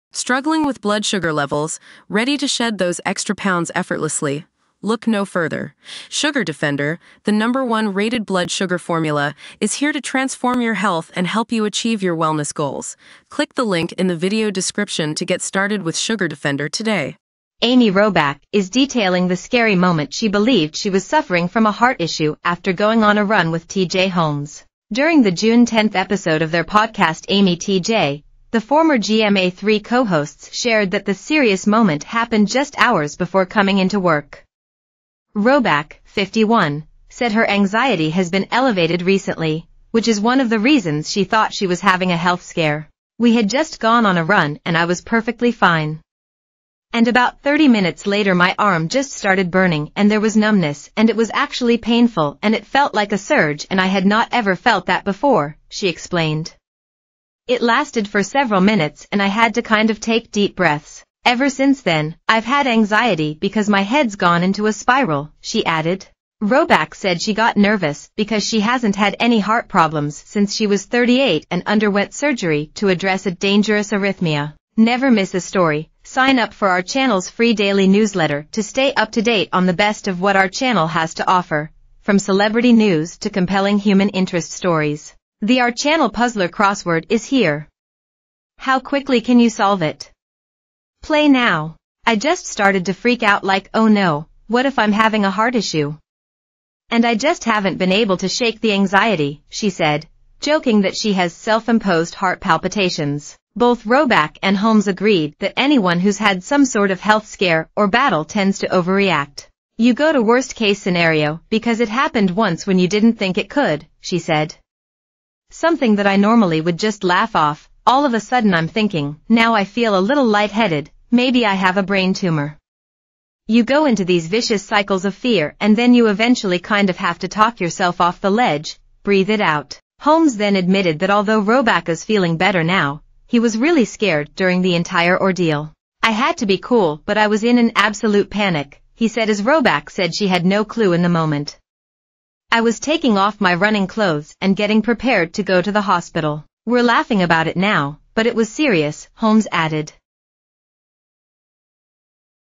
Amy Robach Thought Her Arm Pain Was a Heart Problem, Says Anxiety Causes 'Vicious Cycles of
Amy Robach Thought Her Arm Pain Was a Heart Problem, Says Anxiety Causes 'Vicious Cycles of Fear'